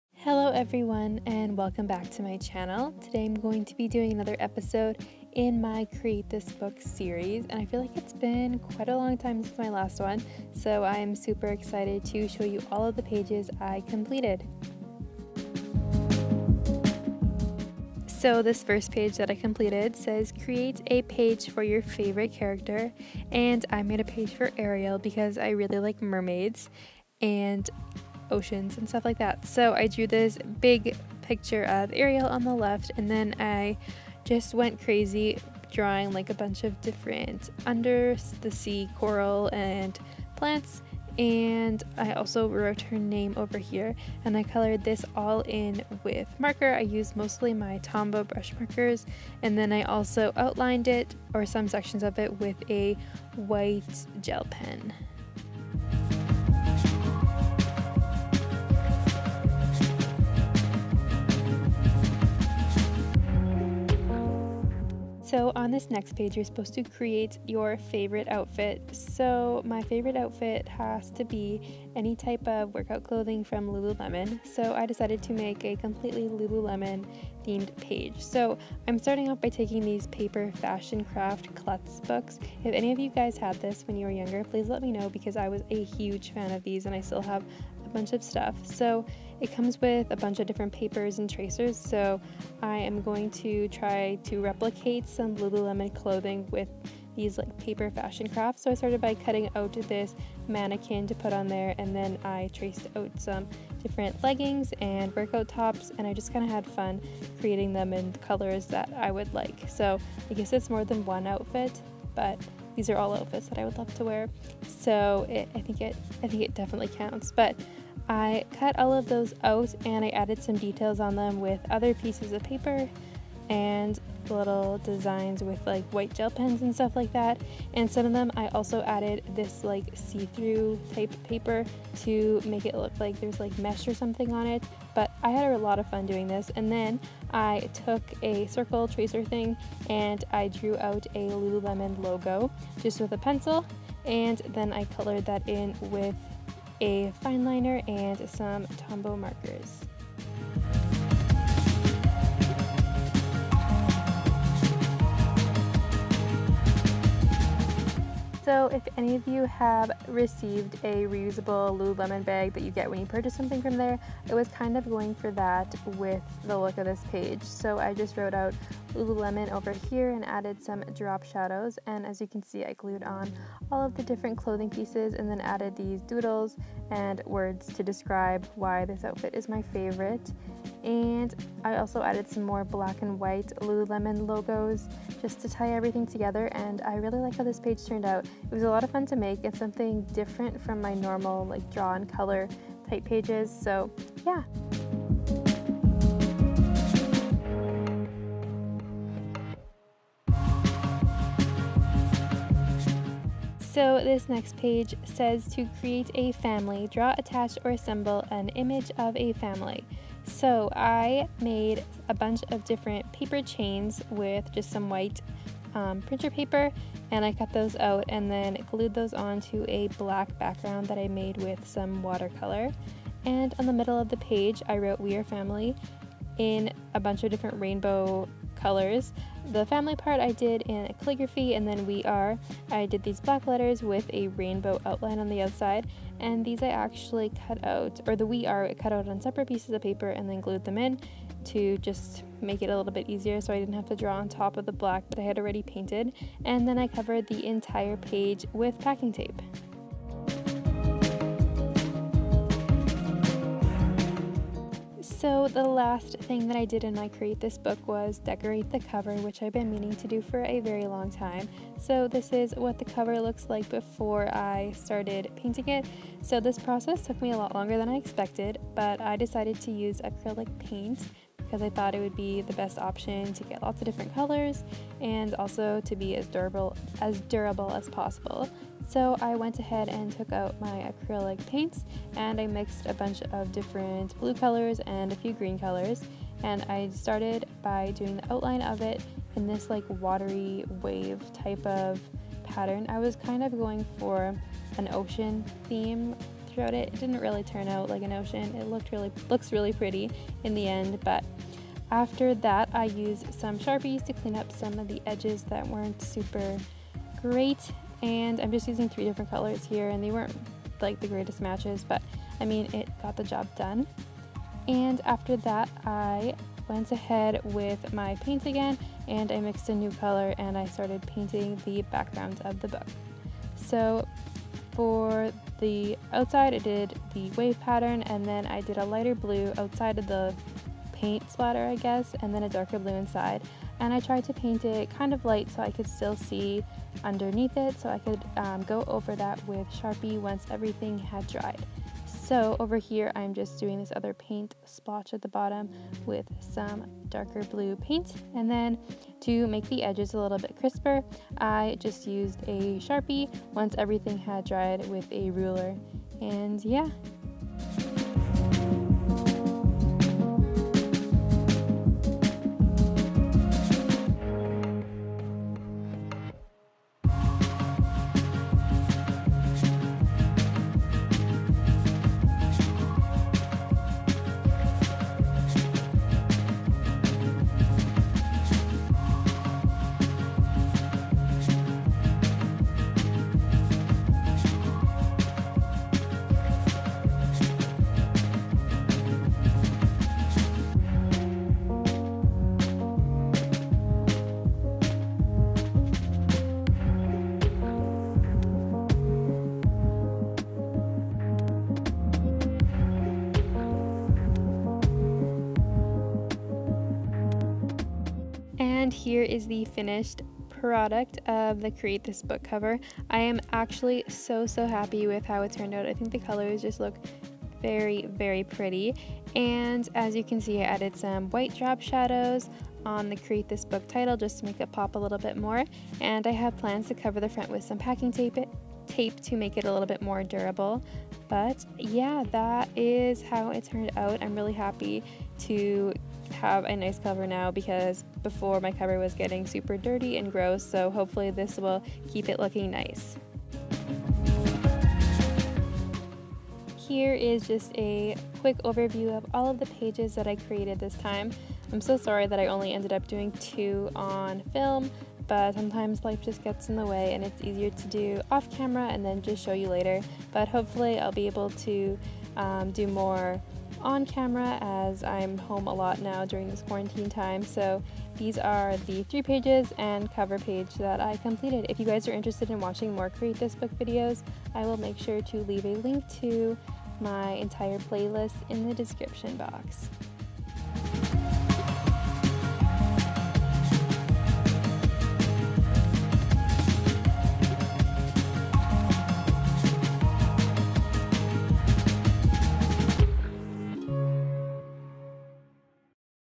CREATE THIS BOOK | episode 6
Hi everyone, welcome to another video:) Today I am showing you how I completed 3 pages in my Create this Book, plus, how I decorated the cover!